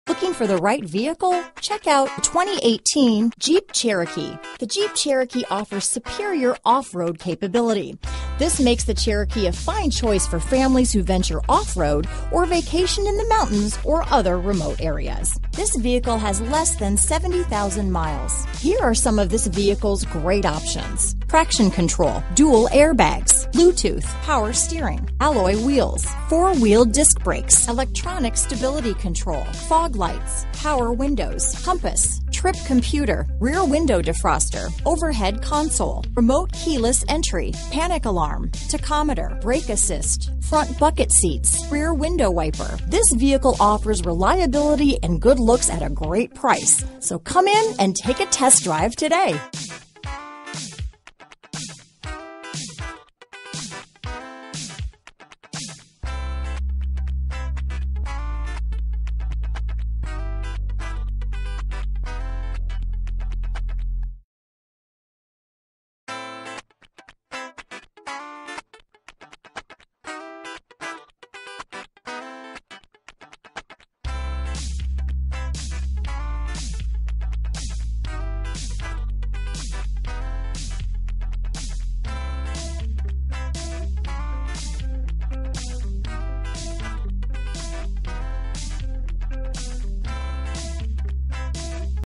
2018 Jeep Cherokee El Paso, Las Cruces, Alamogordo, Ruidoso, Carlsbad MU286692A
Diamond Black Crystal Pearlcoat Used 2018 Jeep Cherokee available in El Paso, Texas at Hyundai of El Paso. Servicing the Las Cruces, Alamogordo, Ruidoso, Carlsbad, TX area.

2018 Jeep Cherokee Latitude Diamond Black Crystal Pearlcoat 2.4L I4 MultiAir 9-Speed 948TE Automatic FWD Bluetooth, Backup Camera, Active Grille Shutters, Altitude Package, Black Day Light Opening Moldings, Cloth Low-Back Bucket Seats, Flex Fuel Vehicle, Front fog lights, Gloss Black Exterior Badging, Gloss Black Fascia Applique, Gloss Black Grille Surrounds, Gloss Black Roof Rails, High intensity discharge headlights: Bi-Xenon, ParkView Rear Back-Up Camera, Quick Order Package 24J, Remote keyless entry, Speed control, Wheels: 18 x 7 Gloss Black 5-Spoke Aluminum. CARFAX One-Owner. Clean CARFAX. 21/30 City/Highway MPGbrbrAwards:br  * 2018 KBB.com 10 Most Awarded Brands